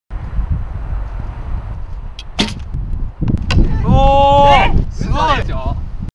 70m先のドーナツの穴を射抜く、なにわのロビンフッド誕生！
パラリンピックの正式競技として1960年の第1回ローマ大会から採用されている歴史ある競技のアーチェリー。障害の種類や程度によって用具の工夫などが認められているものの、リカードオープンのカテゴリーでは70メートル先の的を射ち合う選手の集中力が見どころだ。
真っ直ぐに矢を射ることだけでも十分に難しいなかで、ロビンフッドさながらにドーナツの穴を通すという前代未聞のスゴ技に挑戦。果たして成功なるか！？